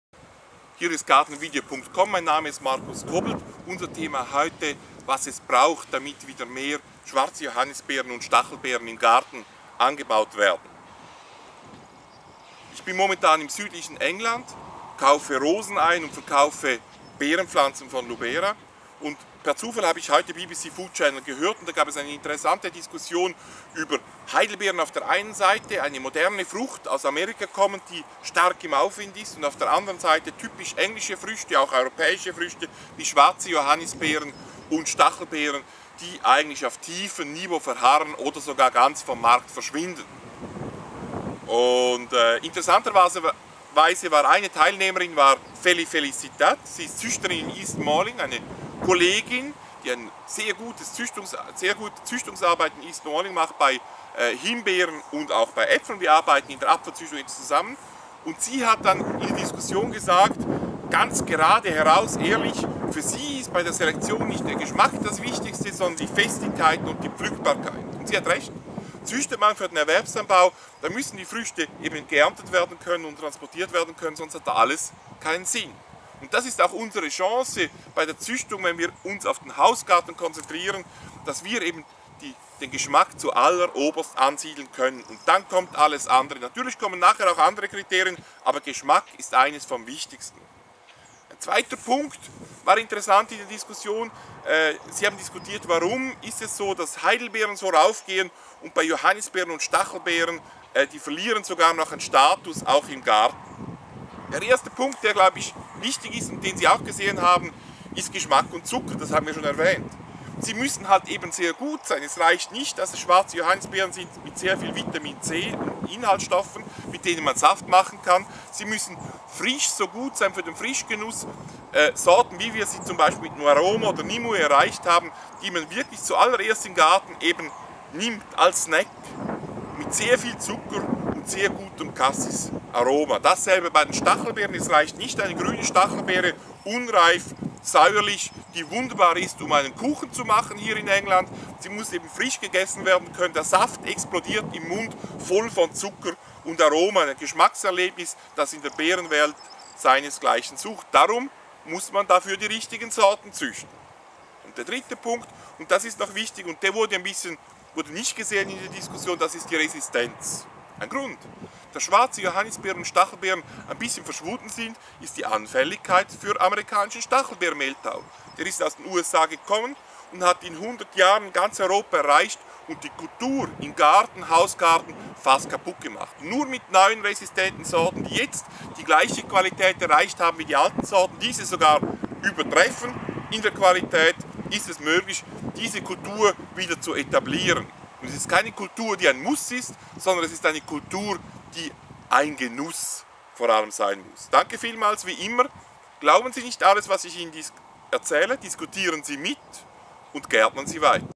Ueber schwarze Johannis- und Stachelbeeren
Markus Kobelt möchte das Image von schwarzen Johannis- und Stachelberen aufbessern und hofft, dass demnächst auch sie in ihrem Garten auf den Geschmack kommen.